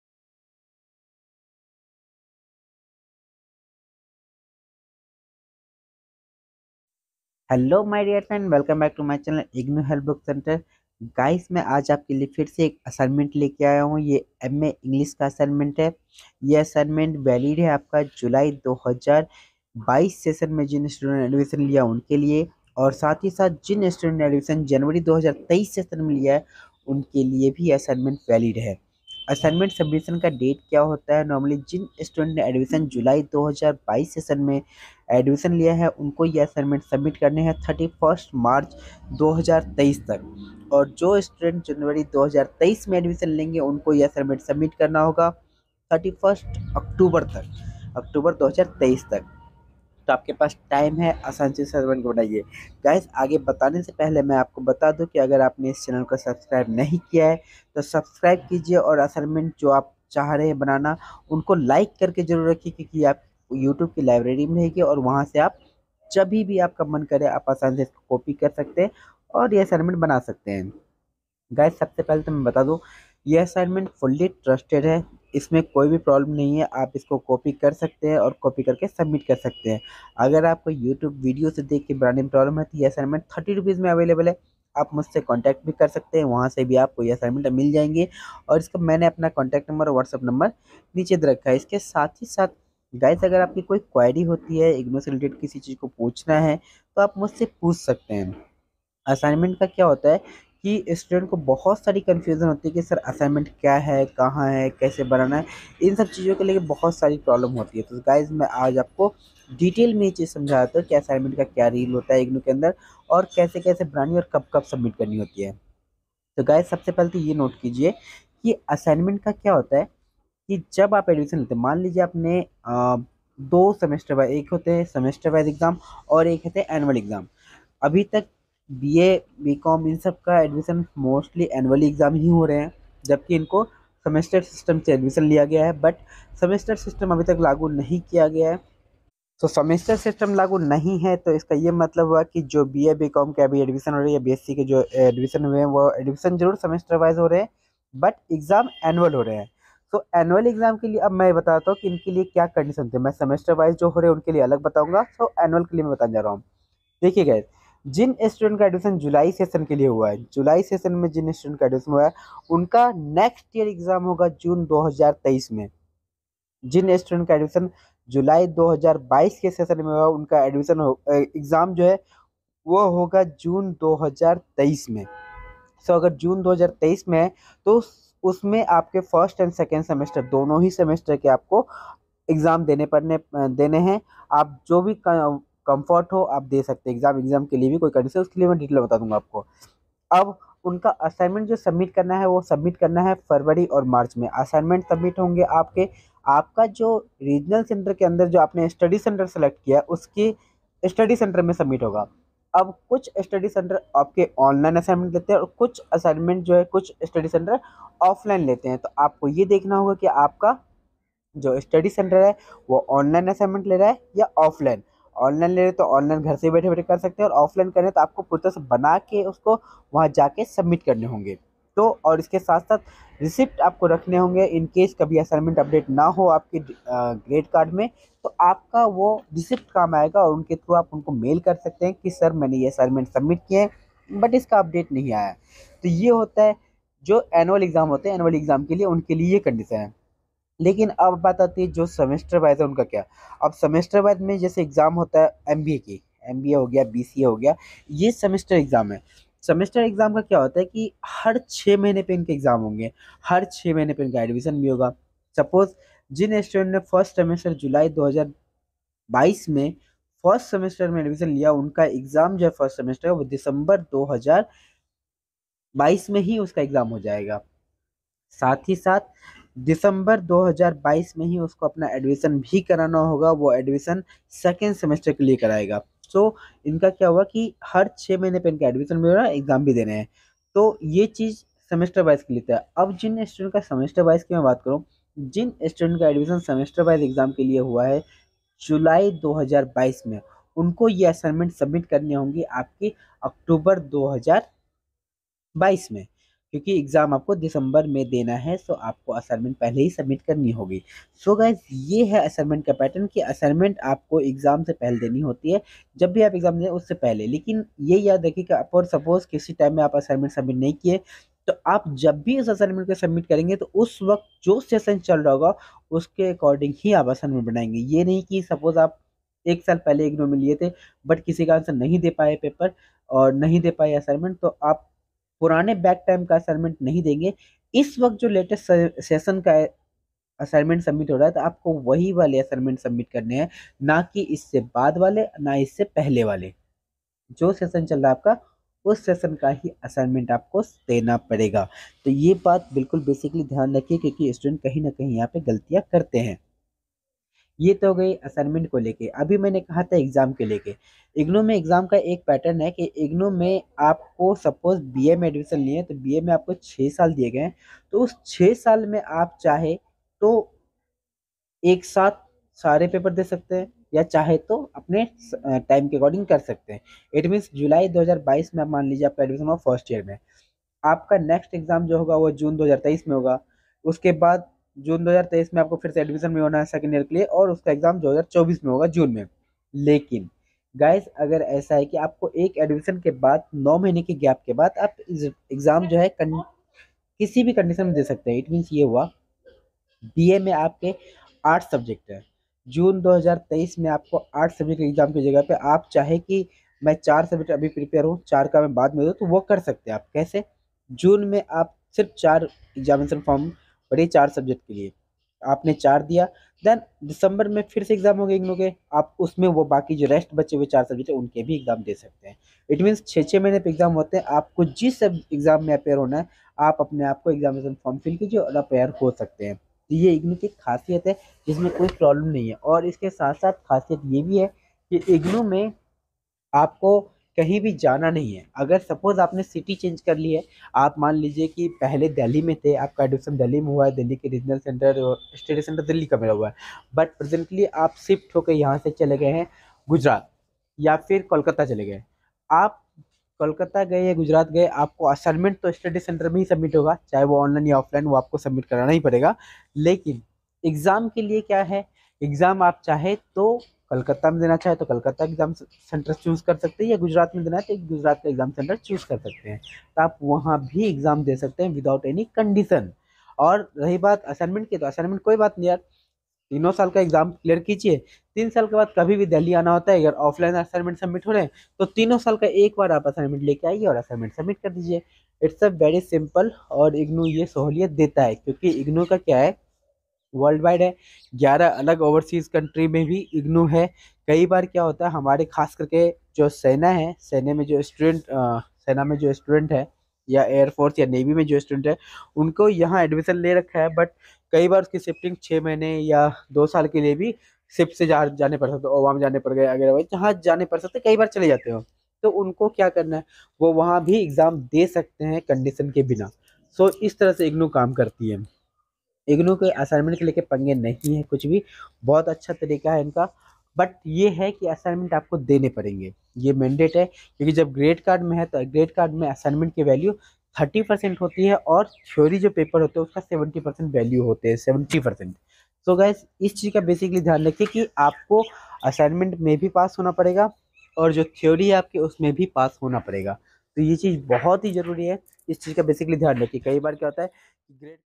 IGNOU MEG-9 Solved assignment for July 2022 and January 2023 session ll Solve Assignments.MA English
IGNOU MEG-9 Solve Assignment for July 2022 and January 2023 session II MA English

This video I shown Ignoh M.A English Honors Solve Assignment for July 2022 and January 2023 session students.

Meg Solve Assignment Link given below